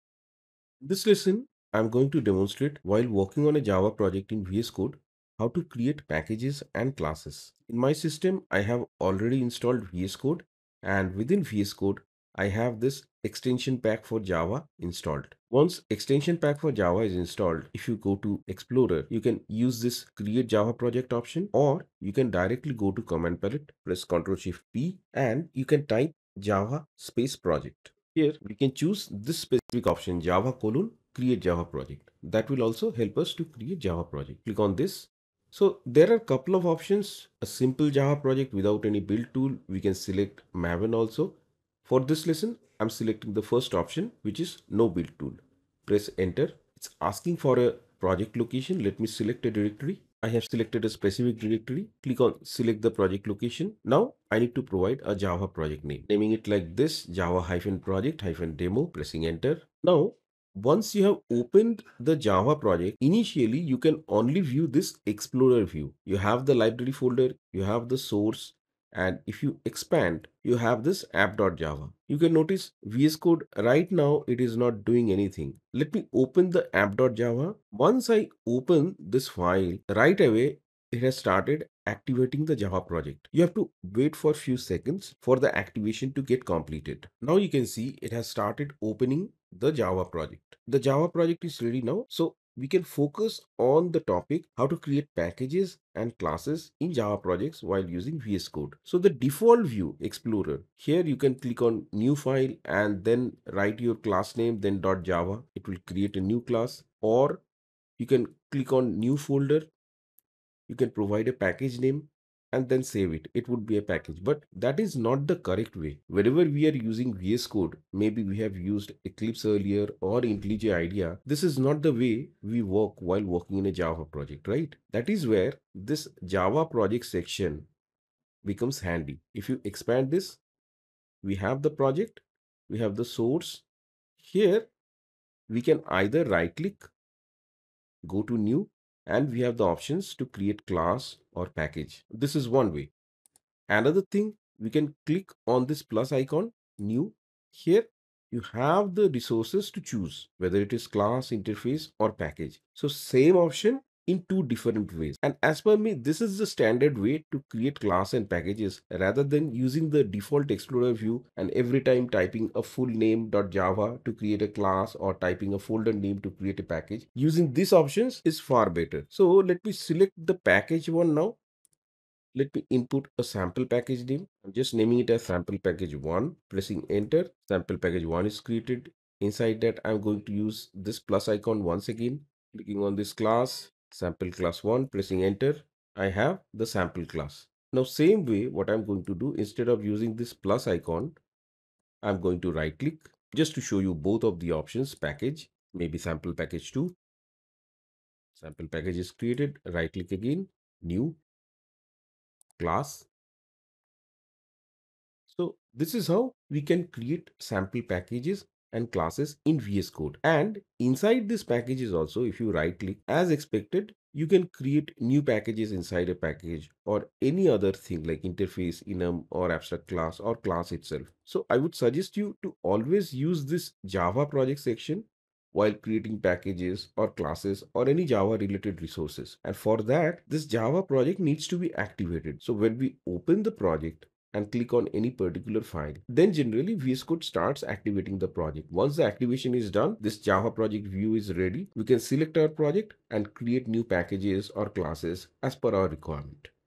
VS Code for Java Beginners: Package & Class Creation | How to Create Package and Class in VS Code
Getting started with Java in Visual Studio Code? In this video, I guide you through the steps to create a new Java project and show you how to leverage the Java Projects view for streamlined development. Learn how to create Java packages, classes, and other resources with ease, avoiding the hassle of navigating through the Explorer section. Perfect for beginners looking to boost productivity in VS Code! 🚀

📌 What You'll Learn:

Setting up a new Java project in VS Code
Using the Java Projects view for package and class creation
Tips to organize your Java resources efficiently 💬 Join Our Slack Community!